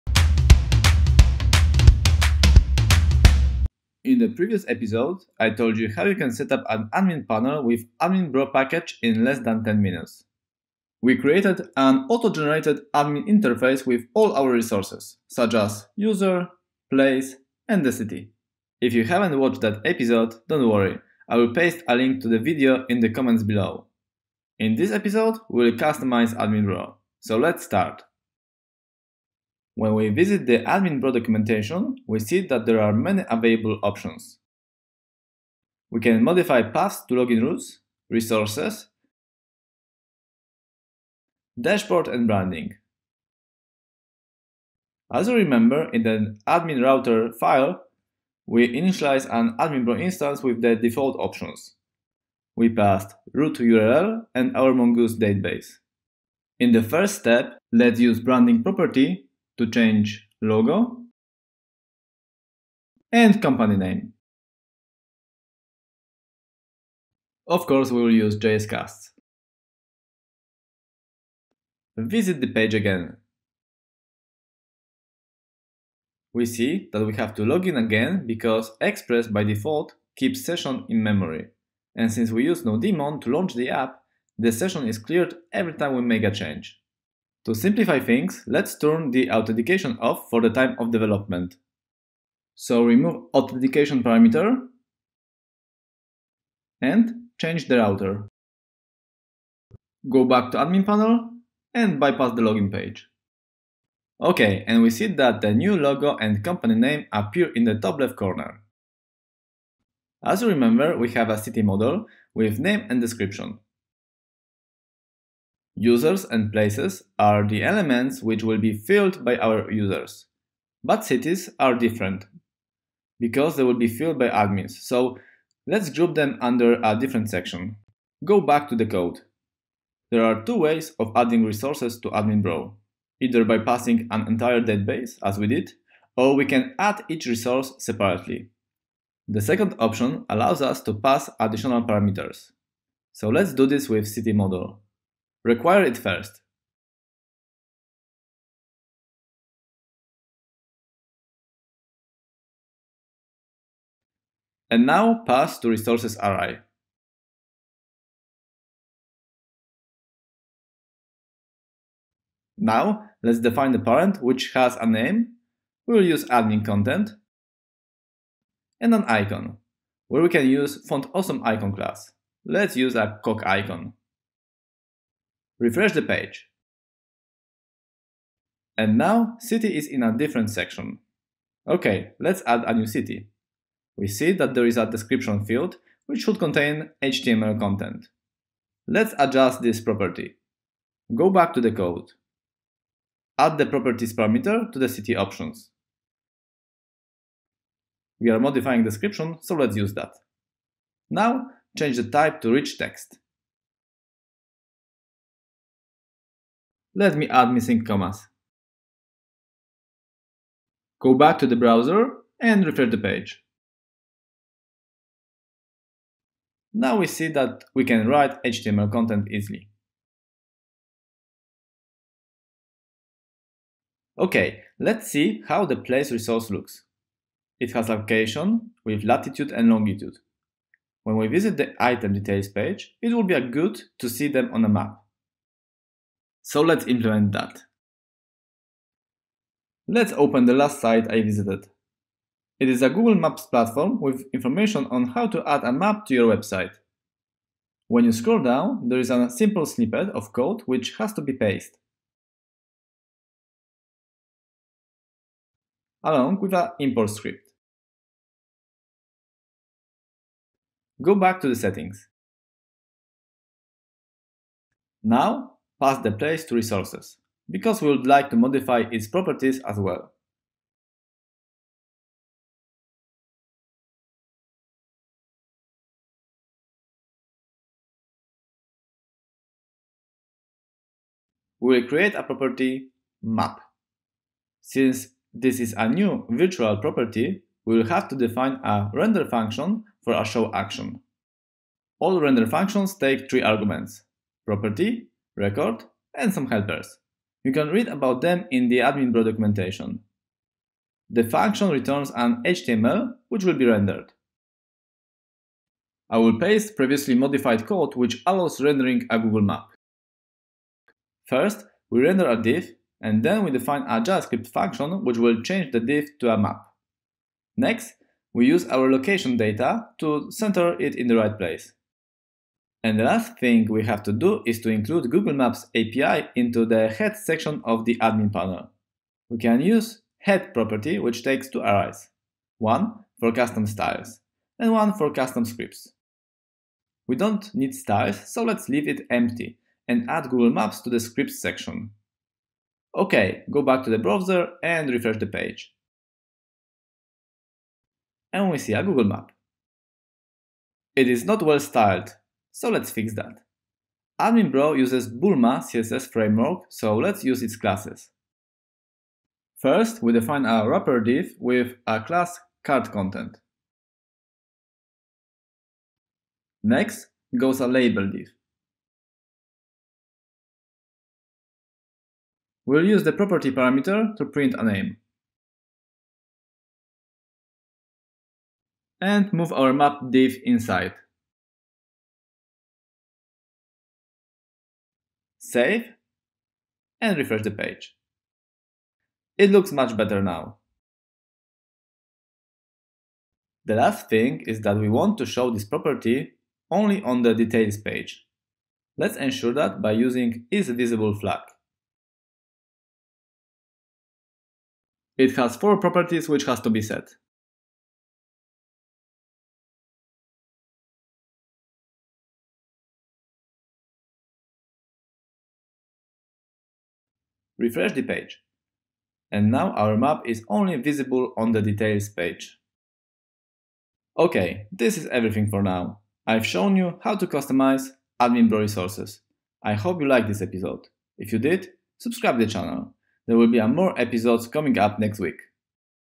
ep11 - adding admin panel to Node.js app - customize resources
In this episode, we will continue working on an autogenerated admin panel by a package called AdminBro We will modify resources.

The full code for this video can be found on our GitHub repository